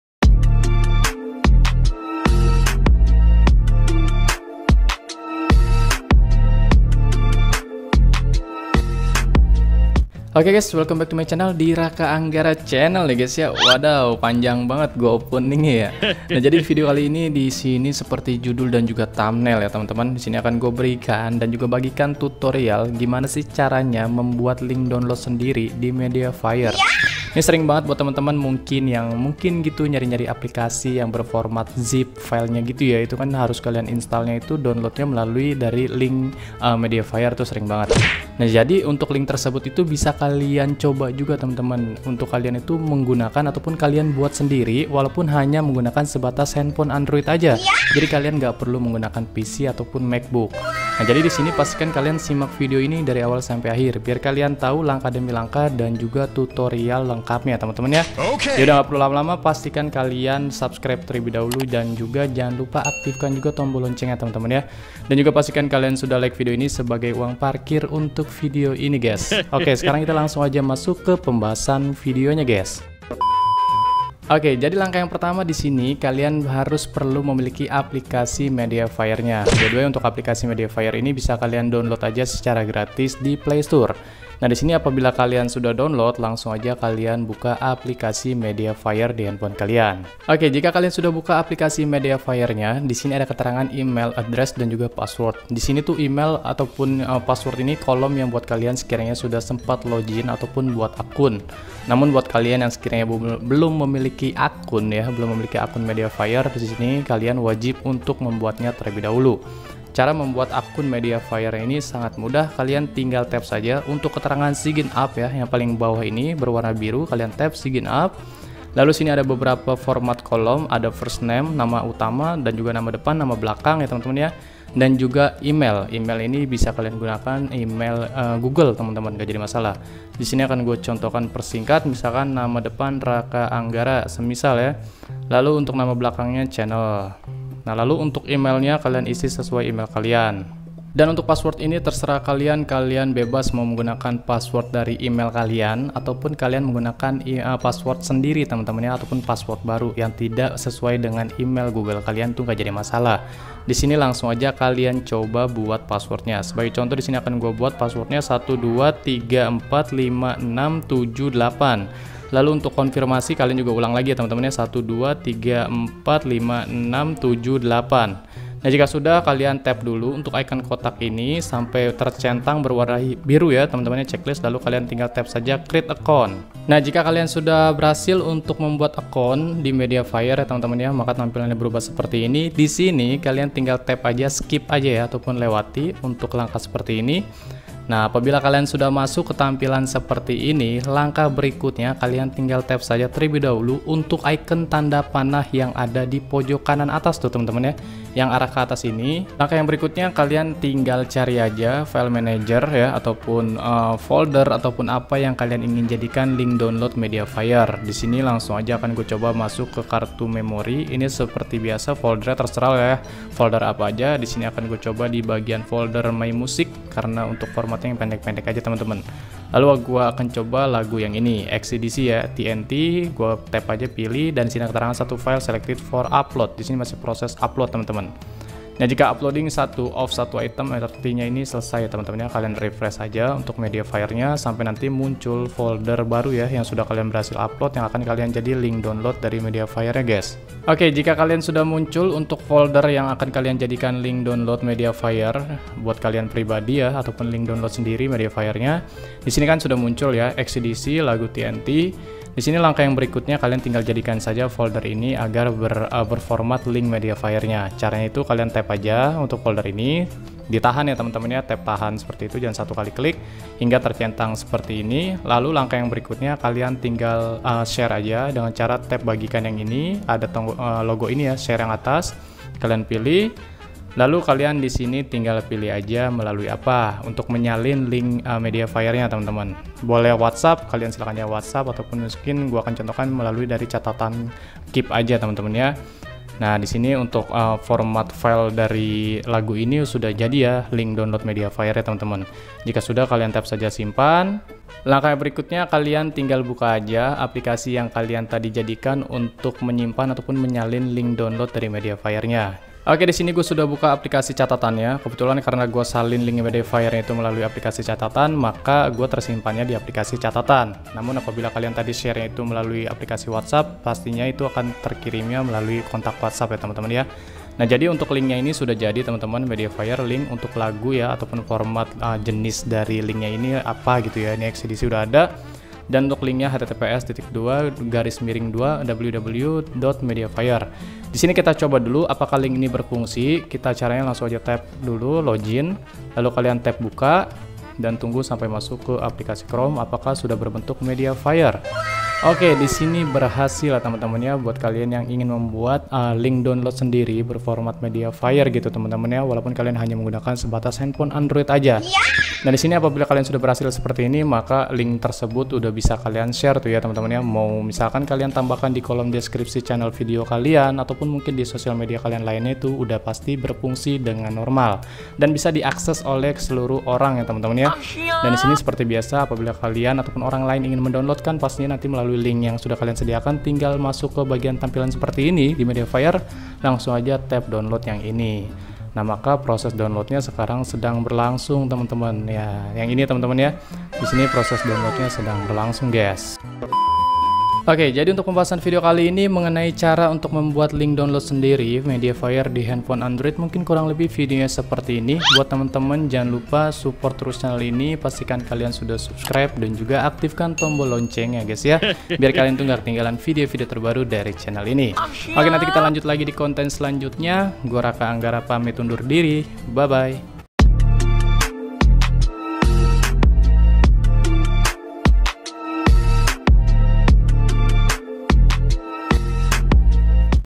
Cara membuat link download sendiri di MediaFire terbaru
Video kali ini saya ingin membahas lengkap mengenai tutorial video cara membuat link download sendiri di MediaFire menggunakan smartphone handphone android.

Sudah tidak asing lagi ya mengenai kata-kata MediaFire. Hal yang sering kita lakukan mengenai MediaFire pastinya pada saat ingin mendownload folder ataupun aplikasi.

Link MediaFire tersebut bisa kalian buat sendiri di Android tanpa harus menggunakan PC ataupun MacBook. Pastinya tutorial ini akan sangat bermanfaat buat teman-teman semua yang belum mengetahuinya. Maka dari itu Pastikan simak videonya dari awal sampai akhir agar tidak tertinggal langkah demi langkah yang sudah saya sampaikan di dalam video ini. Semoga bermanfaat 👋